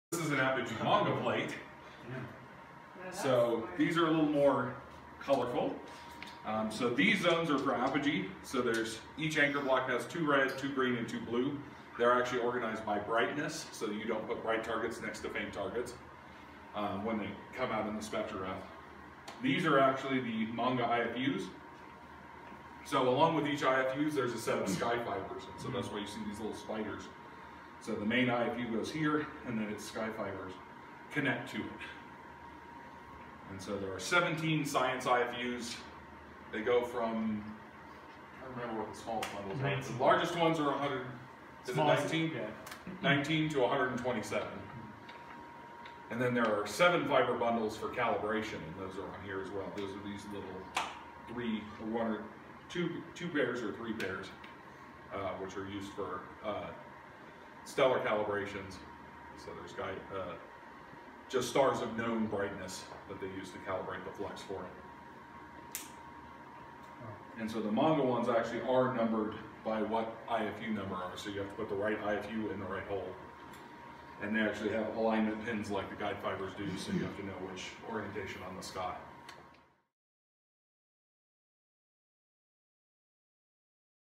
Sloan Digital Sky Survey Plates Explained
Dr. Peter Frinchaboy explains how Sloan Digital Sky Survey plates are used during a 2018 Research for Undergraduate students field trip to Apache Point Observatory.